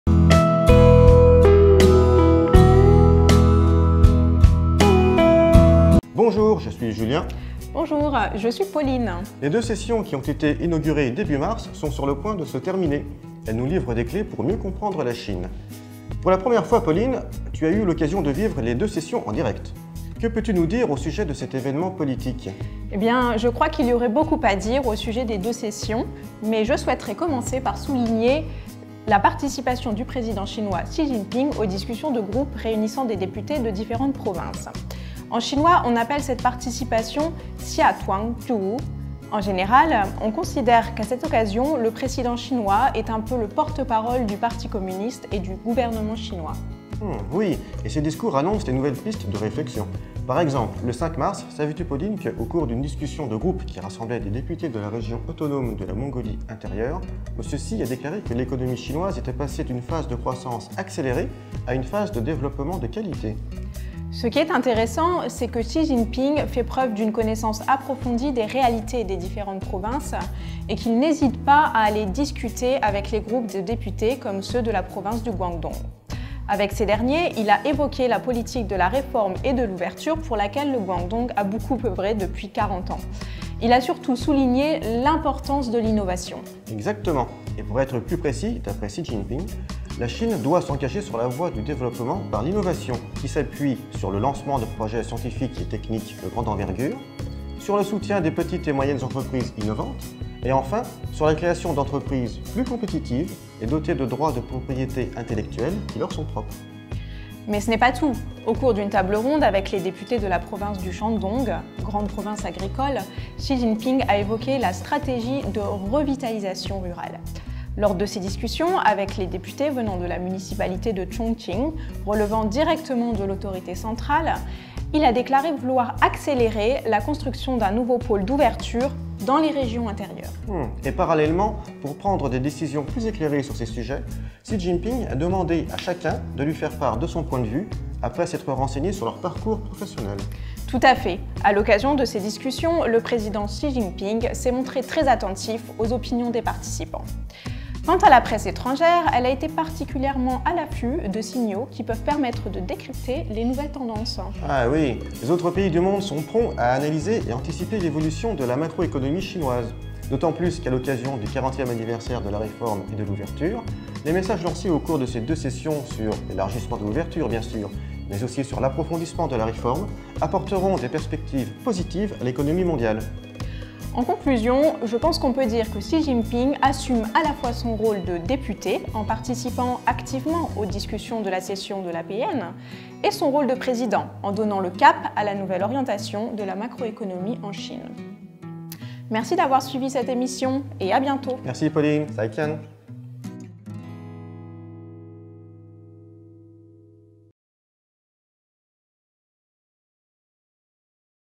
两会观察：习近平“下团组”Les deux sessions : la participation de Xi Jinping aux discussions
三月初开始的中国两会已经接近尾声，可以说两会是了解中国的一把钥匙。中国国家主席习近平在两会期间，参加了不同小组的讨论，用地道的两会用语说，叫“下团组”。习近平主席的“下团组”讲话，也被看作是中国共产党和中国政府工作思路的风向标。
今日中国两位法籍雇员带你从“下团组”看中国未来的经济趋势。
Les deux sessions : Xi Jinping prend part aux discussions
Les deux sessions sont sur le point de se terminer. Elles nous ont livré des clés pour mieux comprendre la Chine. Elles ont aussi montré les résultats positifs de la participation « Xia Tuan Zhu » : des interventions de Xi Jiinping dans les débats au nom du Parti communiste et du gouvernement chinois.